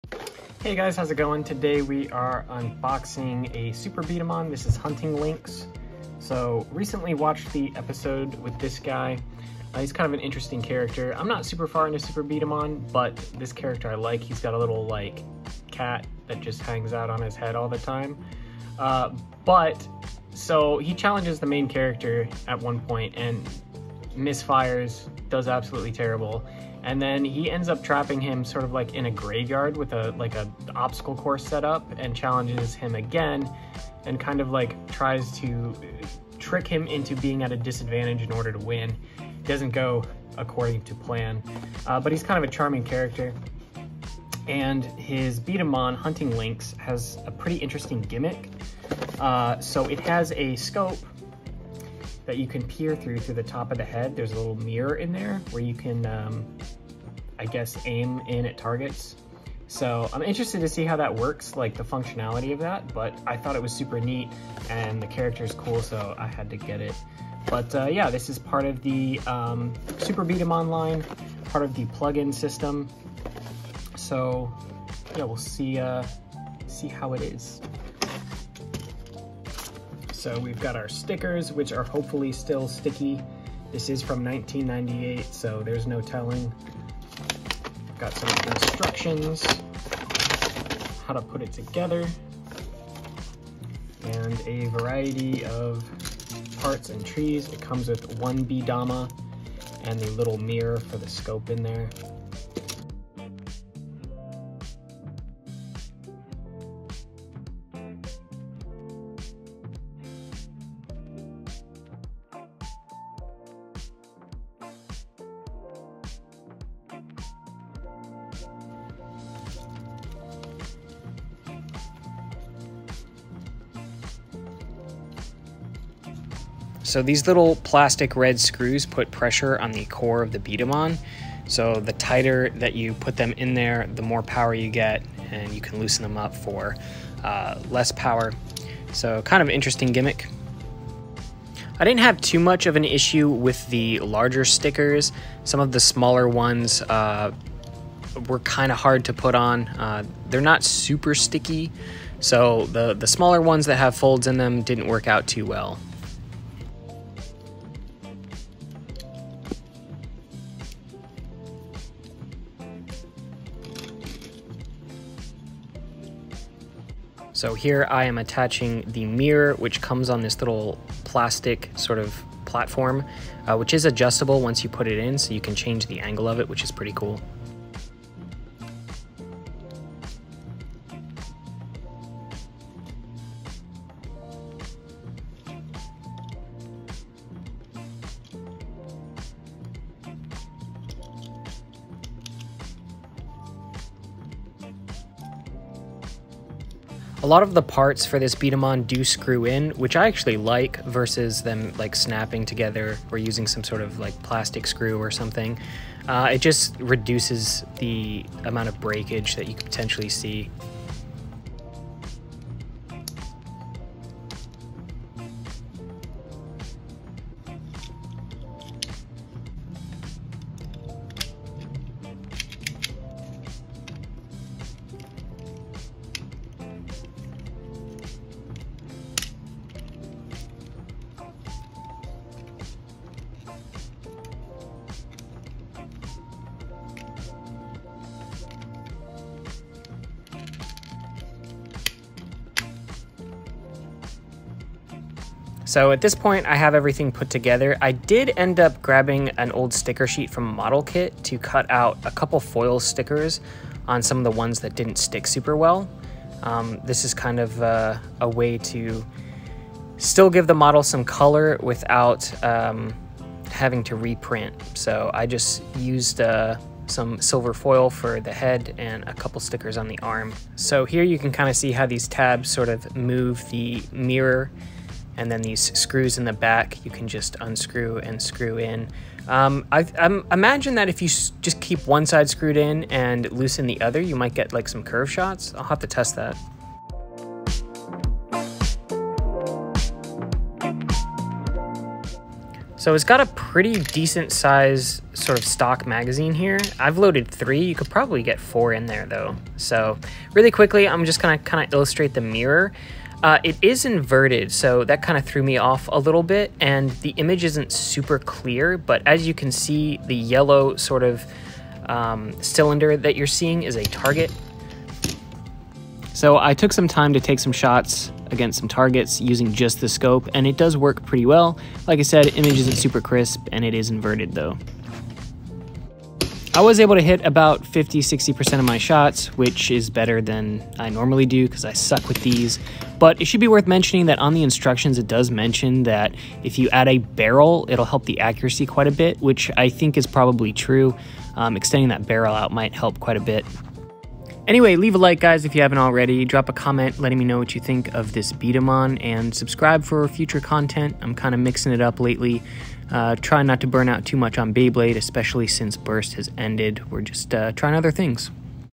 Hunting Lynx Super B-Daman Unboxing Review
Today we are taking a look at one of my favorite super b-daman so far, hunting lynx. This has a working scope!

Target audience 13+

Socials: Nizuma's Beyclub